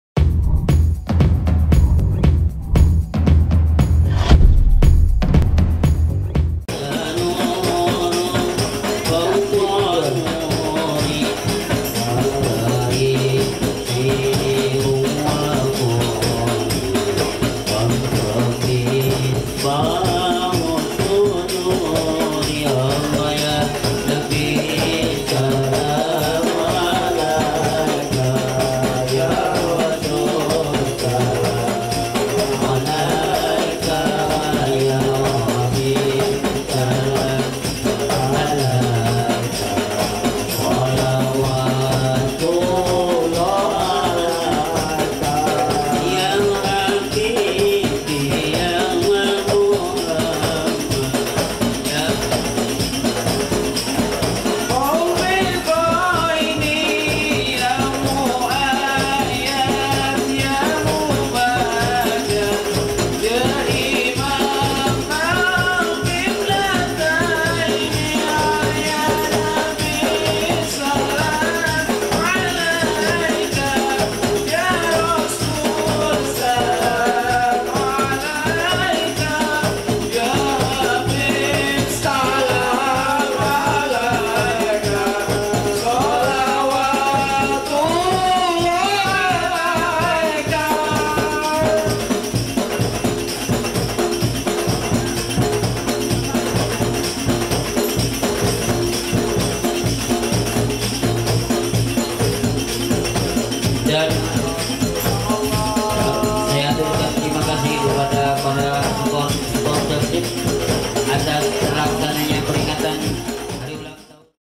Dengerin Sholawatan di iringi musik Tradisional Genjring Talahab Citangtu Kuningan 2017
Video dokumentasi 17 agustusan 2017 di Talahab Citangtu Kuningan